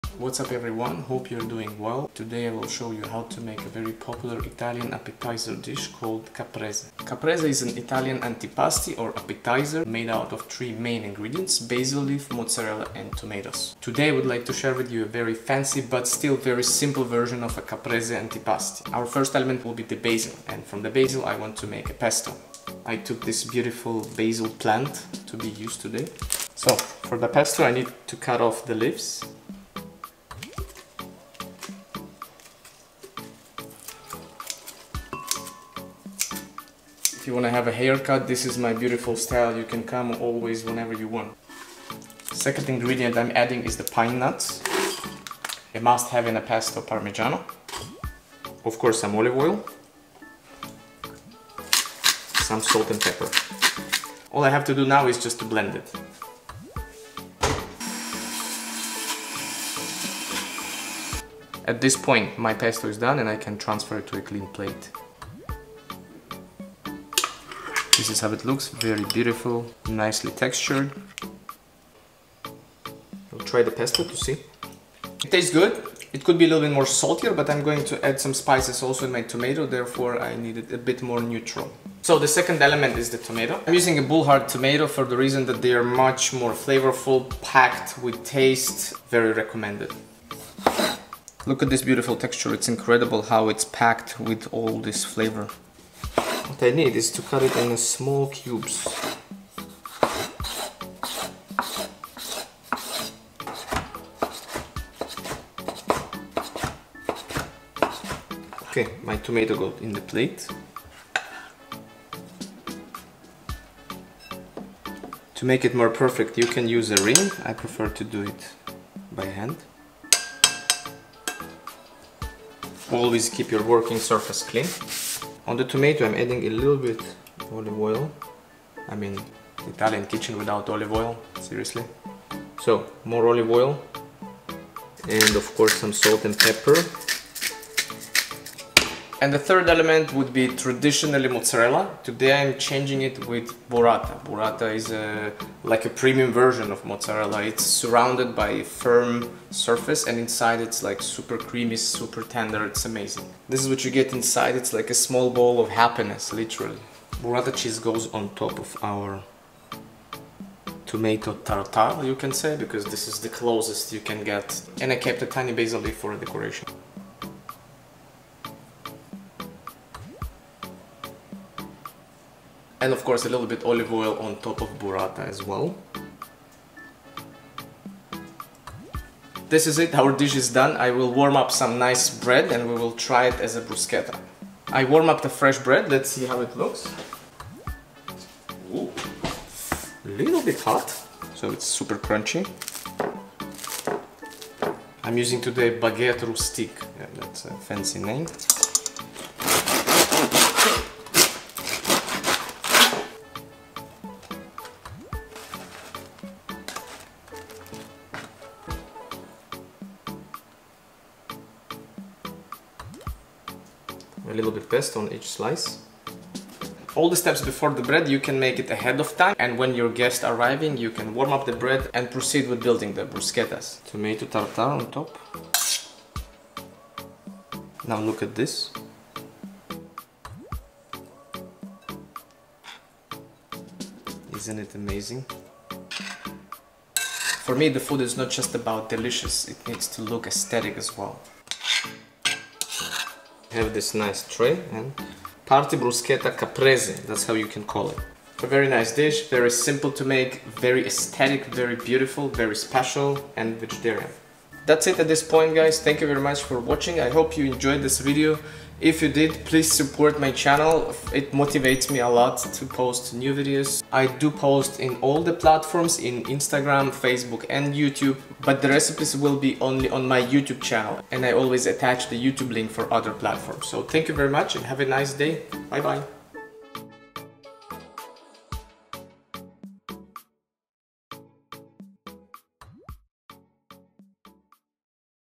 Caprese Bruschetta
This episode is about simple Italian dish called "caprese"
Combination of basil, mozzarella and tomato, well not exactly..
Watch this video to learn how to make a delicious bruschetta caprese, and impress your family or friends on your next event.

My Gear:
► Main camera: Canon EOS R
► Canon 50 mm lens
► Secondary camera
►Motorised slider